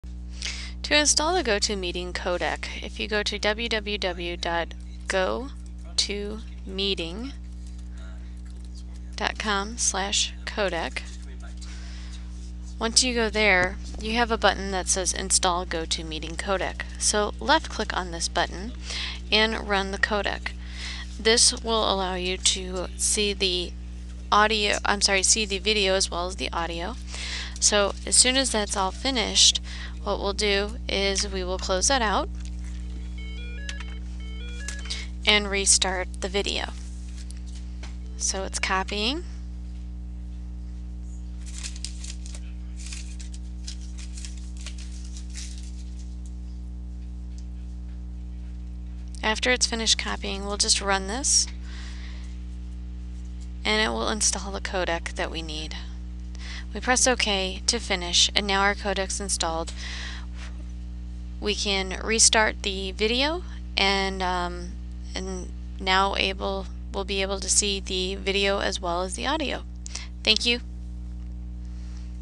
Installing GoToMeeting Codec for your computer
How to install the GoToMeeting Codec to play videos so that you can see the video and hear the audio together.